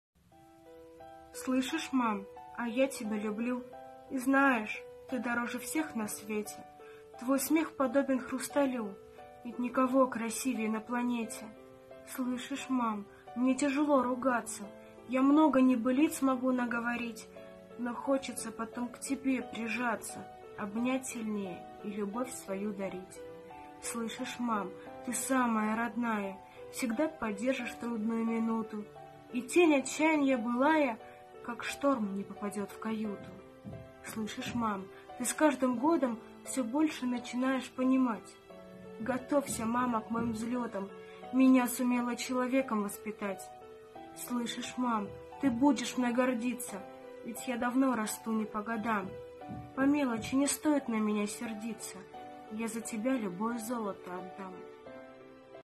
Слышишь, мам, а я тебя люблю I Автор Ирина Пилипейко
Читает автор Ирина Пилипейко (instagram @iriz_zka)